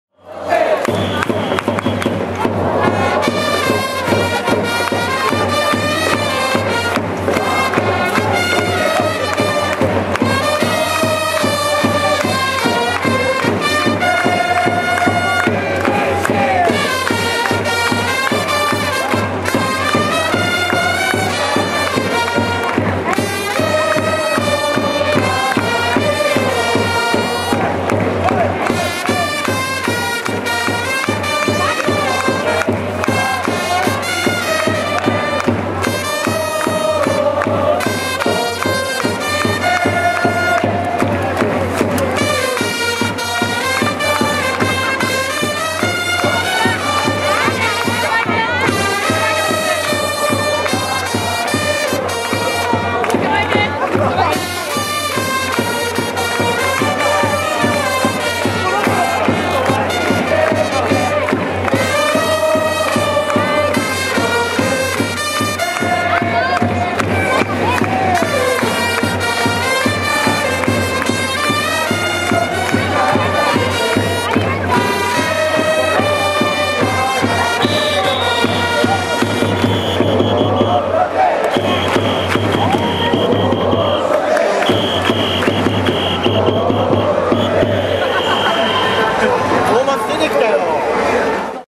2011/07/30 マリンフィールドの風
コーデック変えたら大丈夫なのかな？
日時：2011年07月30日
試合：千葉ロッテマリーンズ　８　-　７　東北楽天
会場：QVCマリンフィールド

マリンでもこんな幸せな時代があったのですよー。
今ではあり得ない（笑

子供向けの動画らしいです。